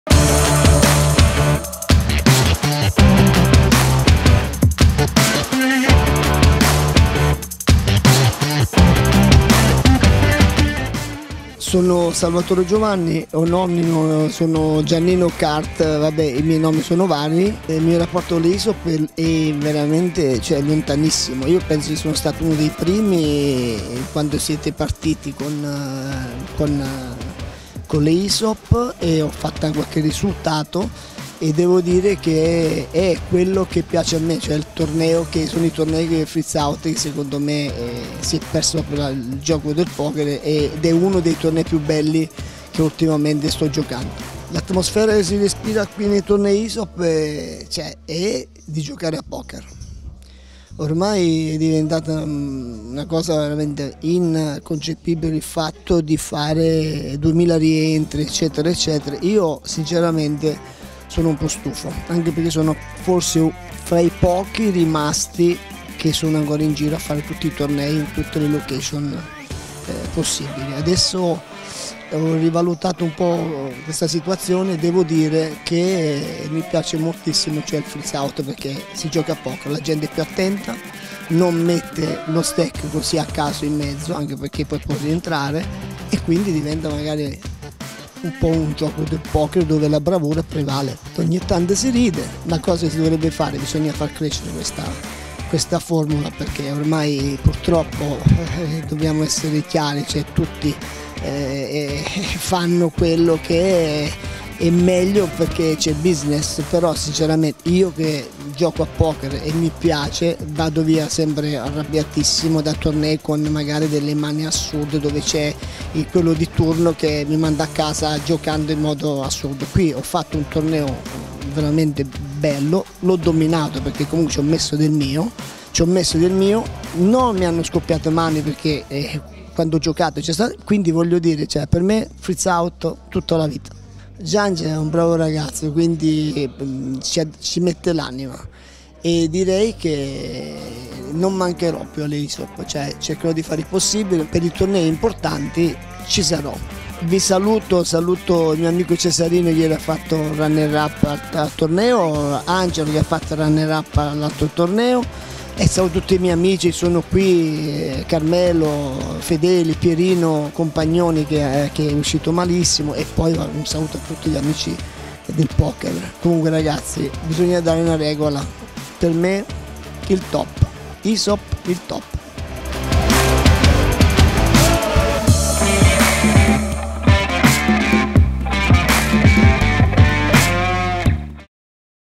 Freezeout è il vero Poker parola di GianninoKart Giovanni Salvatore
Freezeout il vero Poker parola di Giovanni Salvatore GianninoKart ai Campionati Italiani 2019.
Amico di vecchia data (quasi 10 anni) del circuito Italian Series Of Poker™, GianninoKart durante i Campionati Italiani 2019 racconta di come alle ISOP riesca ancora a trovare l'atmosfera del vero poker, grazie alla formula freezeout che permette alla bravura del giocatore di prevalere. Buona visione!